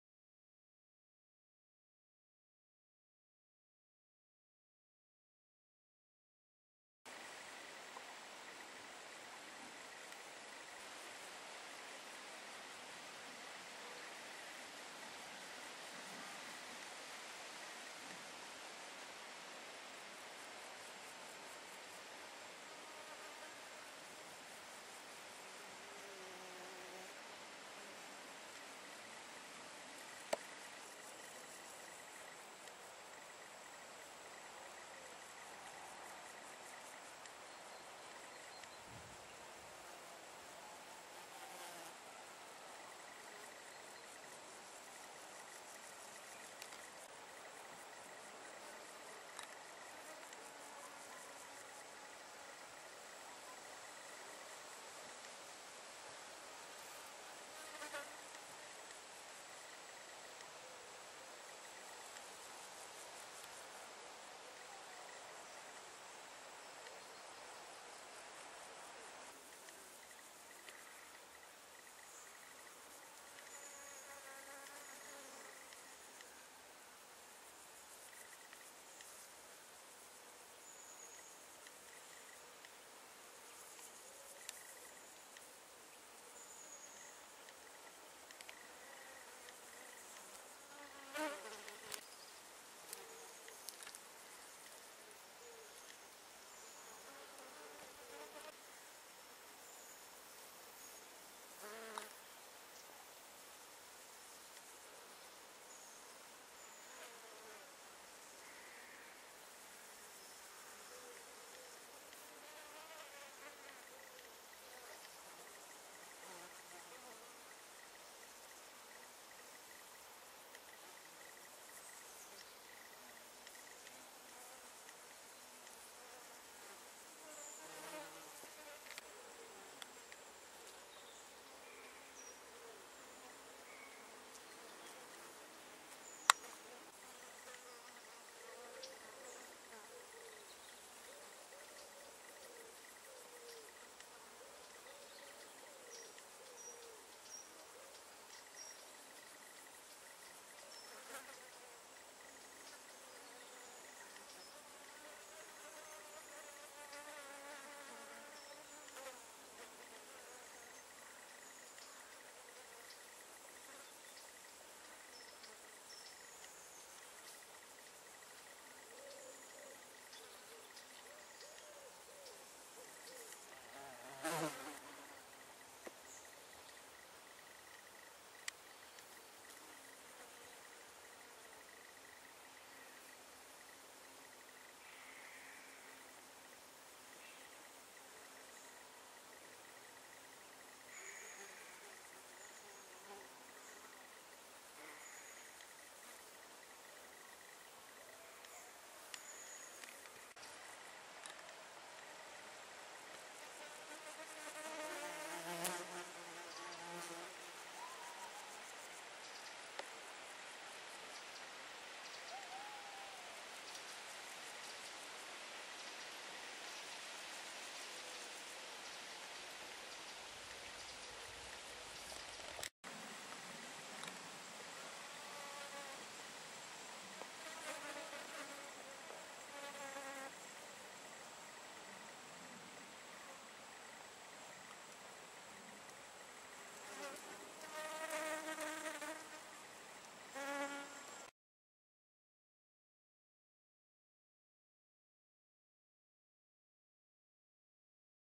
Southern White Admiral Butterflies Limenitis reducta
The handsomely marked Southern White Admiral Butterfly Limenitis reducta is known as Le Sylvain azuré in France where this film was taken. Its range substantially overlaps with that of the similar White Admiral Limenitis camilla in Europe. The Southern White Admiral is absent from the UK.